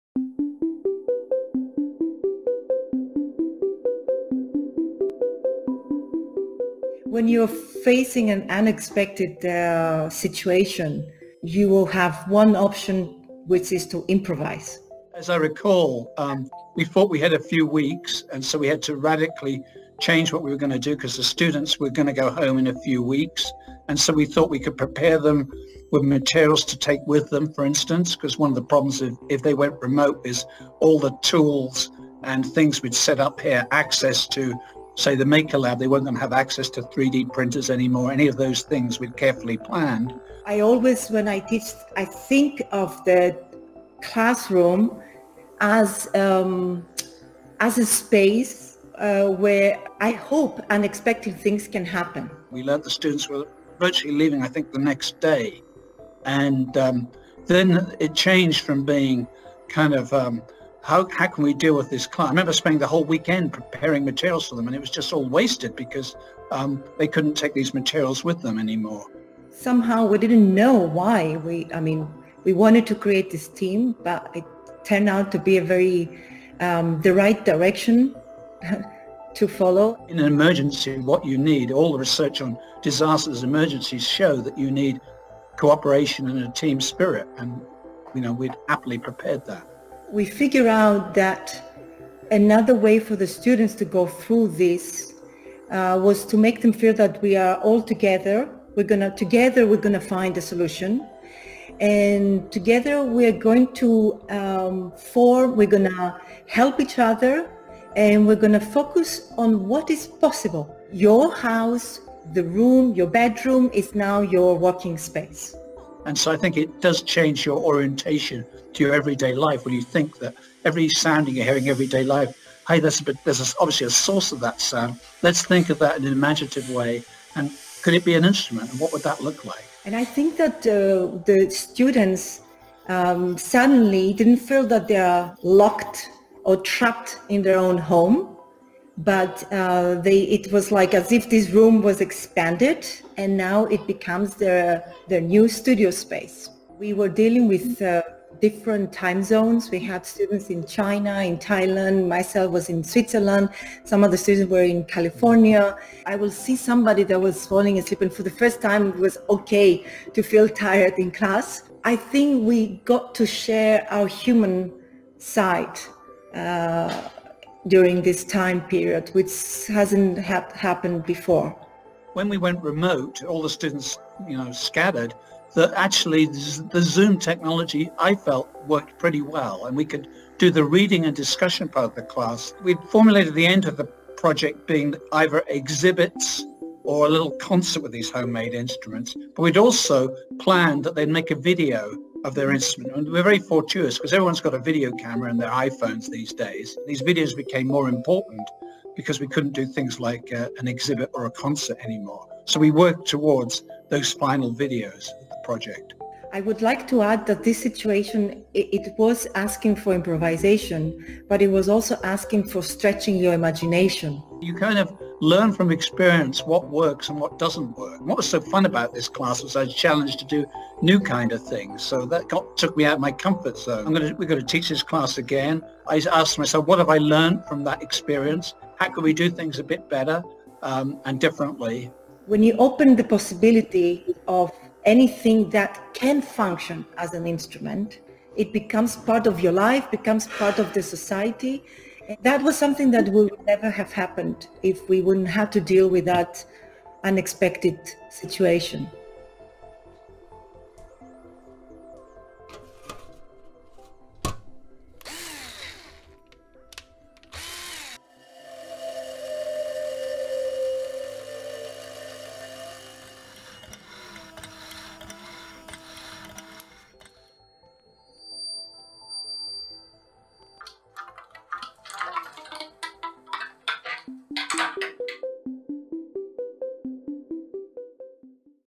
Bending Instruments Innovation Project
Marianthi Papalexandri-Alexandri, College of Arts and Sciences, Cornell University
Trevor Pinch, College of Arts and Sciences, Cornell University

Trevor Pinch and Marianthi Papalexandri-Alexandri were awarded a grant to develop a class in which students create an instrument and use it to explore aspects of sound and performance. As the move to remote learning during the pandemic limited access to materials and workshop tools, Pinch & Papalexandri-Alexandri asked students to be creative and use their own living spaces and materials they had on hand to complete the assignment.